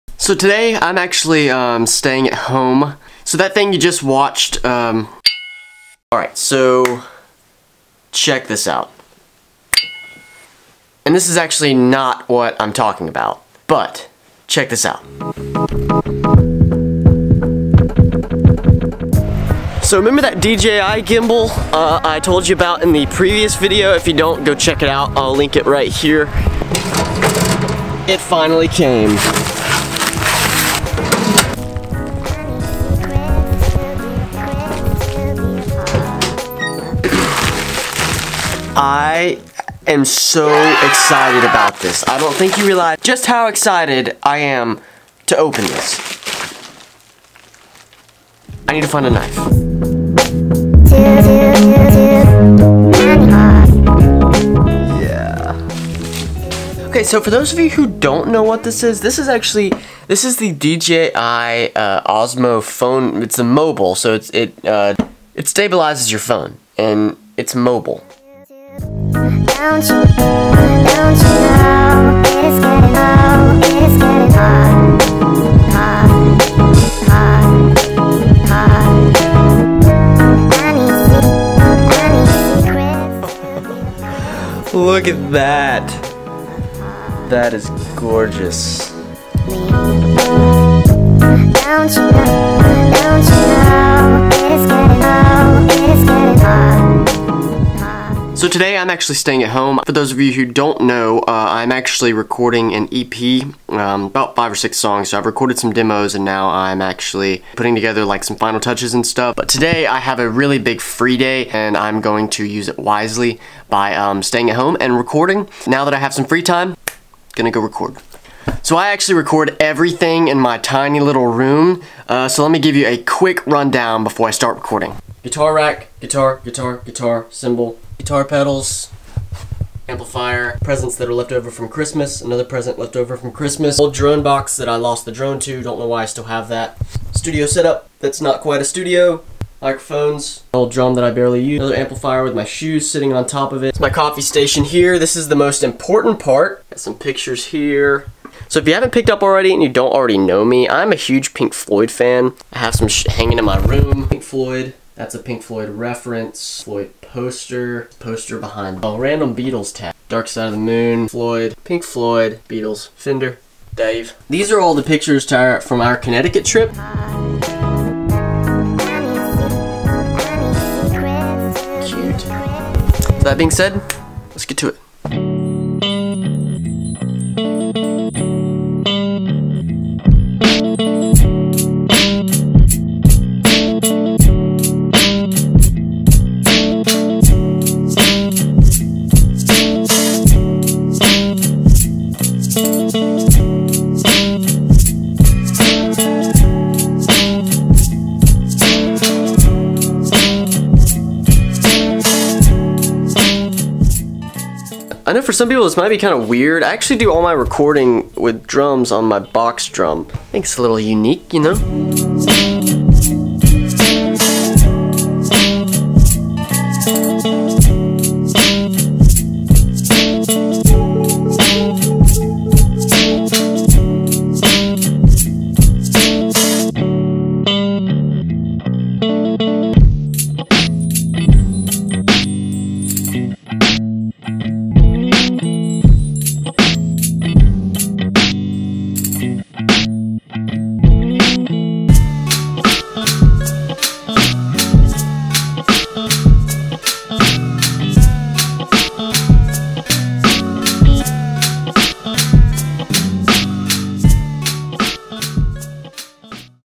it FINALLY CAME!
What seemed like a long-awaited arrival... it was only 6 days / also finishing up an EP I've been working on!

Filmed this video in 1080p on this

Bendy Tripod

Attachment for Joby Gorilla Pod